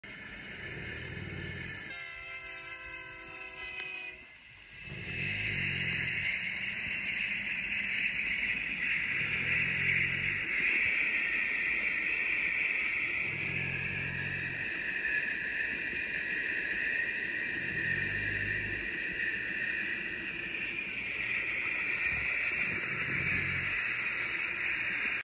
A Union Pacific sd70m and 2 amtrak p32-8bwhs leads amtrak 11 with 2 private cars
Coasterfan2105